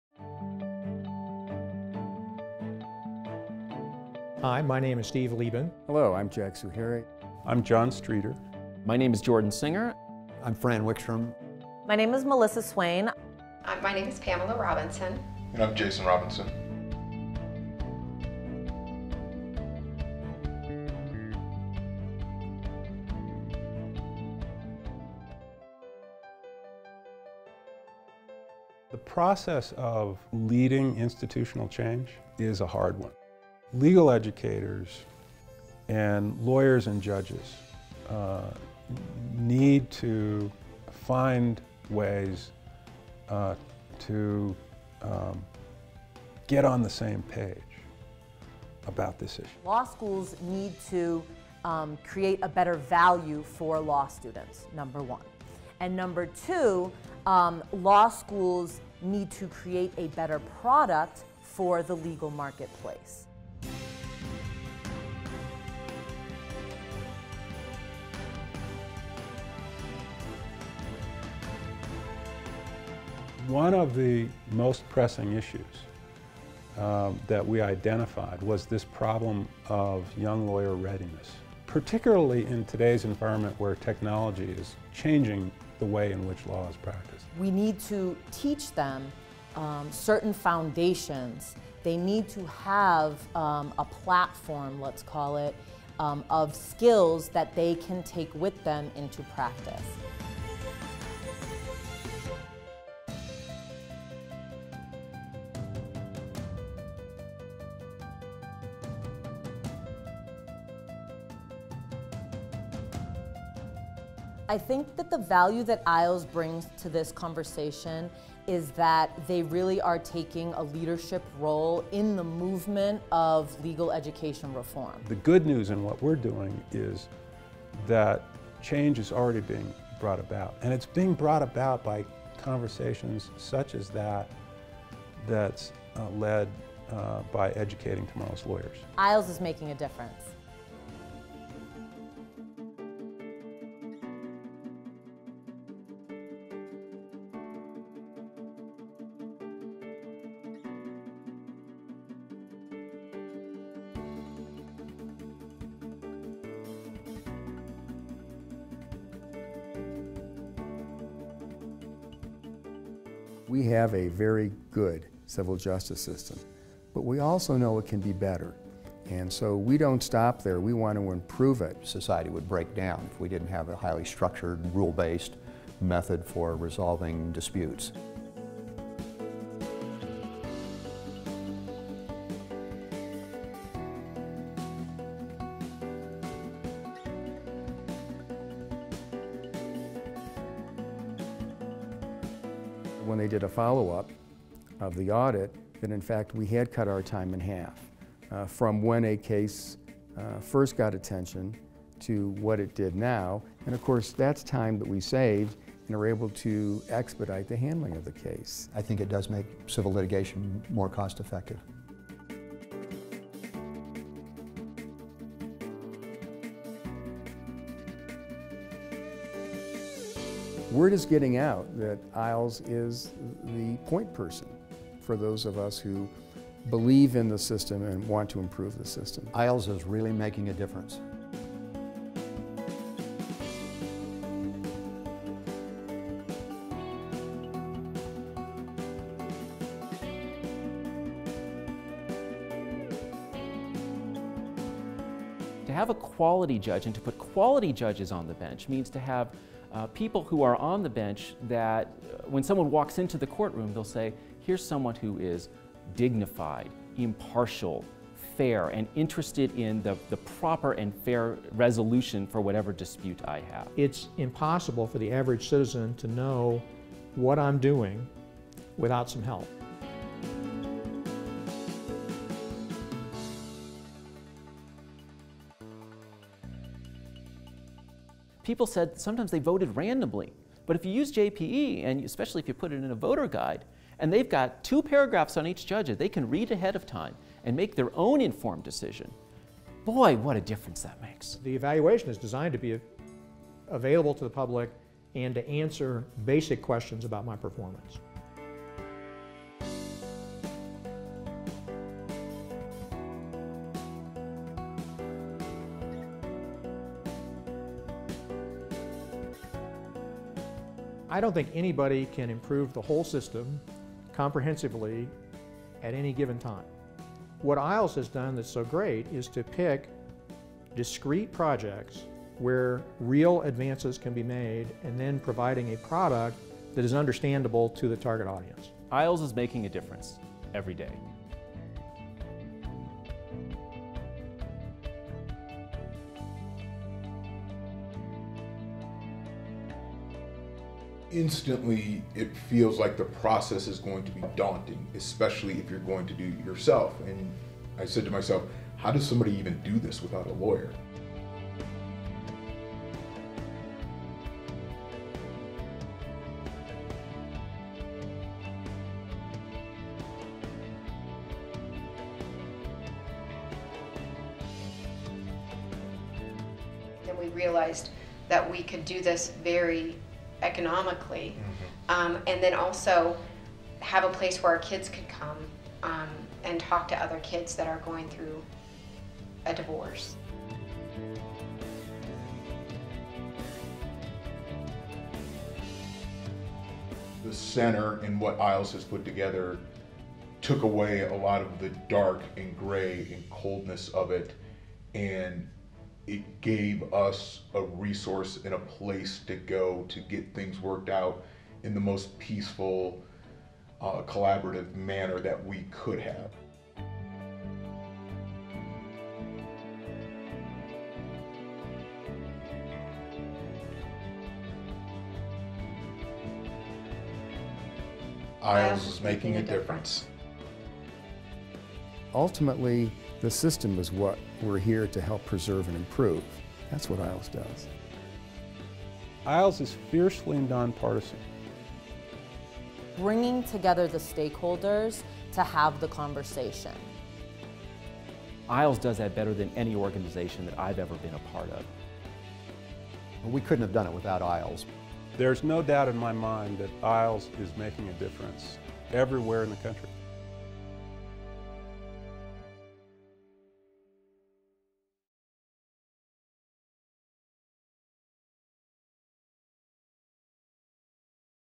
IAALS: National Impact
IAALS is making a difference. From coast to coast and everywhere in between, we have collected stories from real people who share IAALS' passion for and dedication to a legal system that is accessible, trustworthy, relevant, and responsive. Their personal accounts of impact are woven throughout this video. Advancing innovation and excellence in our courts and legal profession is a challenging goal, and measuring impact and outcomes can be even harder. But, the evidence is clear. In every year since our founding in 2006, our impact is real, it is important, and it is happening on a national scale.